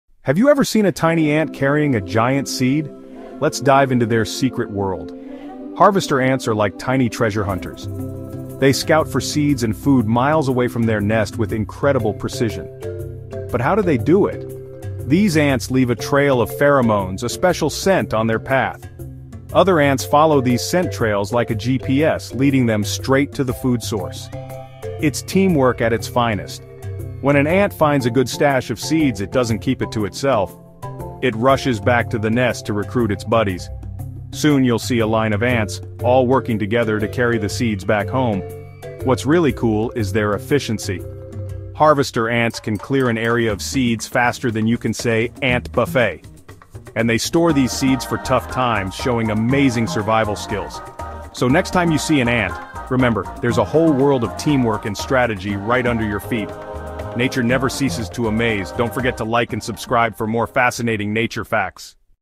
The Secret Life of Harvester Ants: Nature's Tiny Foragers
explore the fascinating world of Harvester ants in "The Secret Life of Harvester Ants: Nature's Tiny Foragers." This one-minute film unveils their remarkable hunting behavior, showcasing how these tiny but mighty foragers gather seeds and food for their colony. With clear narration, viewers will learn about their intricate teamwork, navigation skills, and the vital role they play in the ecosystem. Watch as we highlight their unique physical traits—from their robust bodies to their formidable mandibles. Join us on this captivating journey into the hidden lives of these industrious insects!